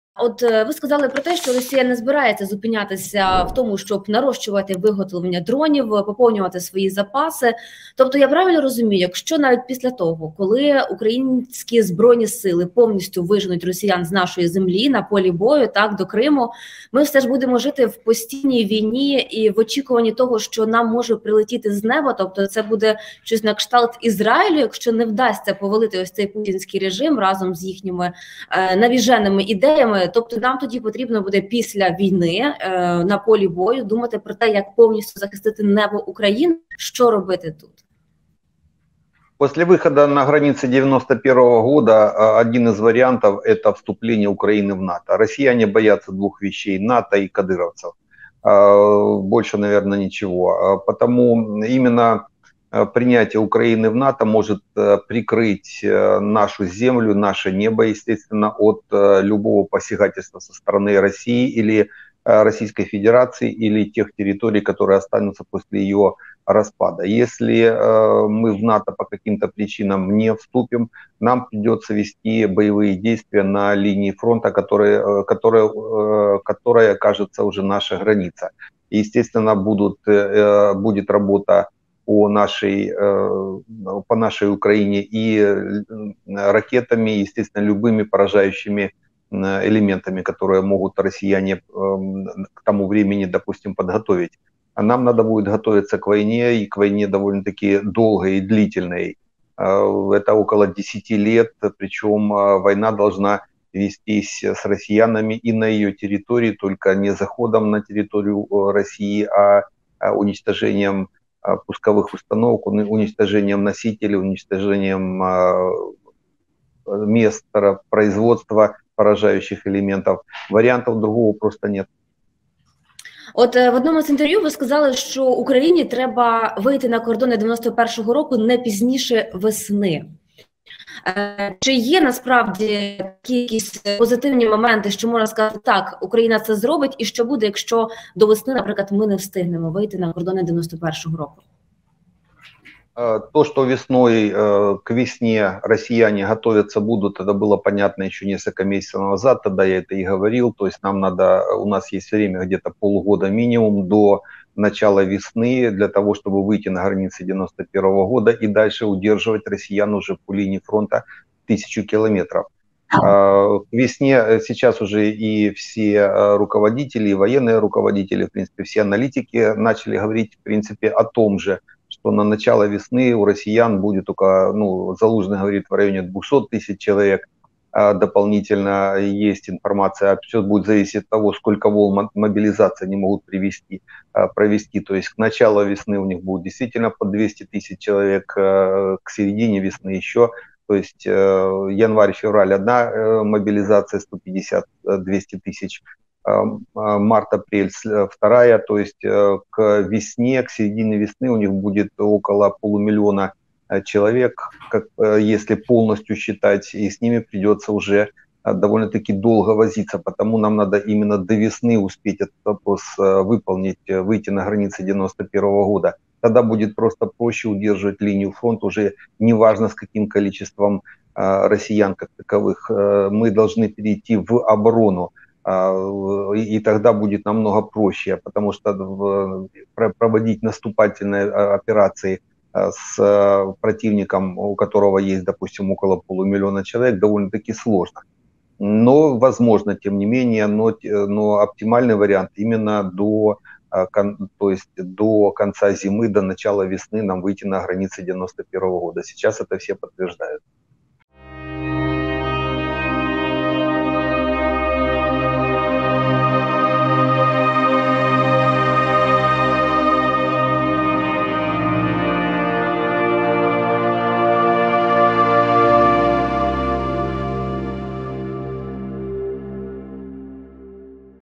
❗️ВІЙНА З РФ БУДЕ 10 РОКІВ! Є тільки ОДИН СПОСІБ уникнути цього – Світан
Військовий експерт Роман Світан упевнений, що звільнення усіх окупованих територій України не закінчить війну з Росією, оскільки постійно будуть йти бої на кордоні, а вся територія обстрілюватиметься ракетами і це триватиме дуже довго. Вихід лише один…